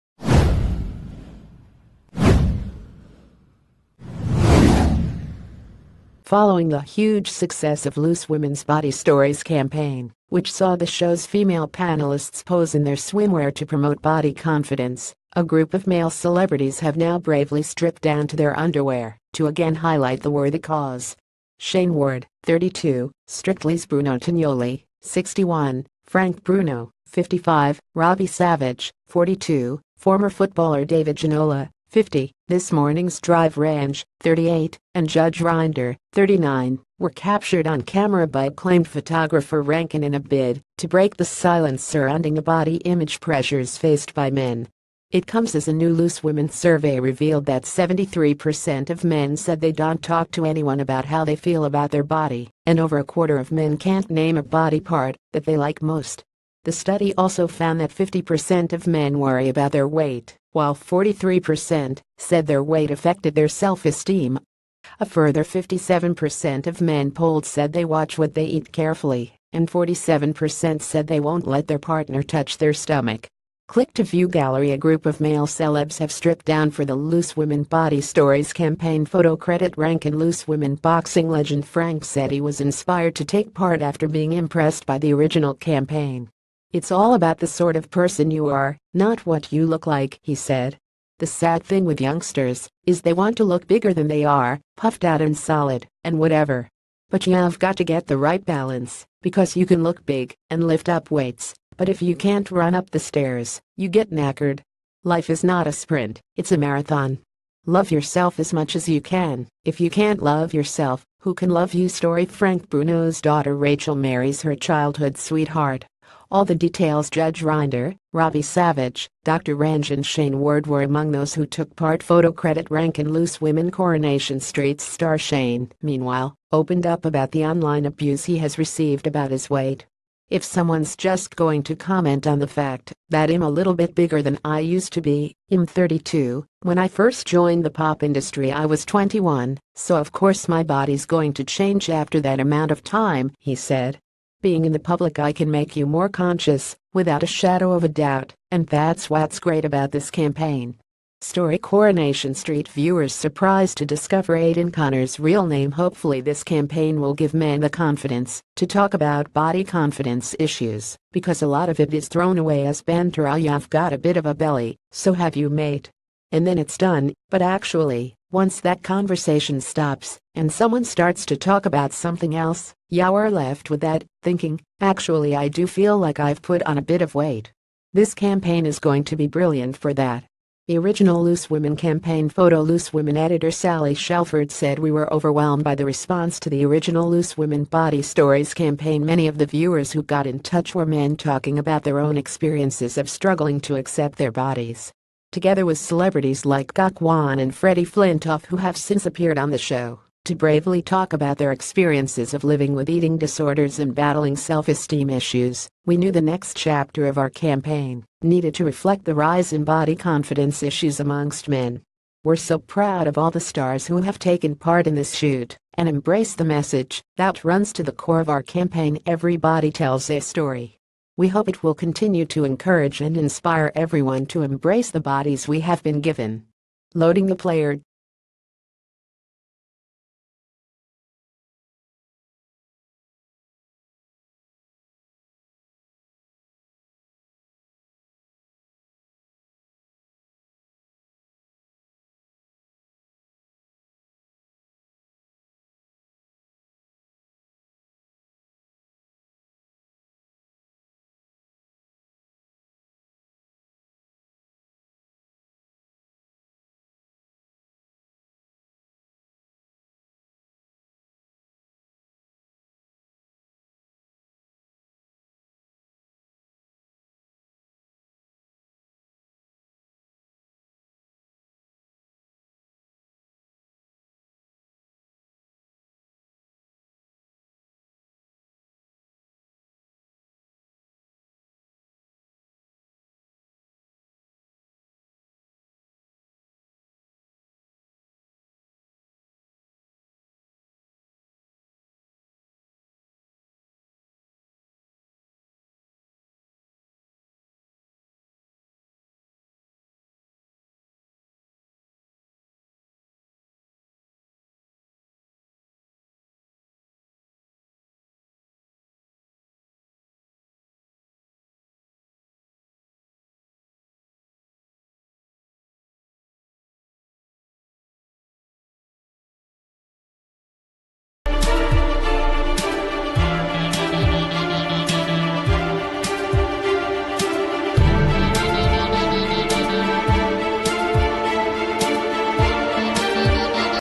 Breaking News | Male celebrities strip off for loose women body stories campaign
Male celebrities strip off for loose women body stories campaign

Following the huge success of Loose Womens Body Stories campaign, which saw the shows female panelli...